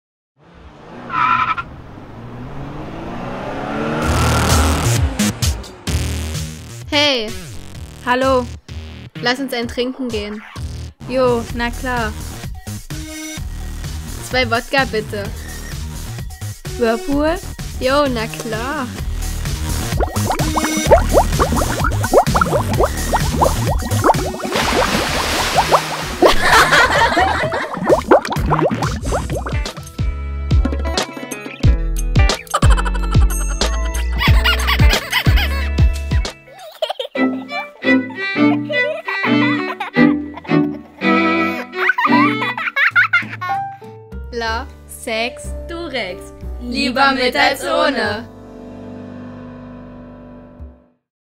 Schneckenspiele
Ein Abend voller Sinnesfreuden bleibt manchmal für immer in Erinnerung. Ausgedacht und produziert von interessierten Besucherinnen des Point Jugendtreff in Lieskau. (9. August 2013)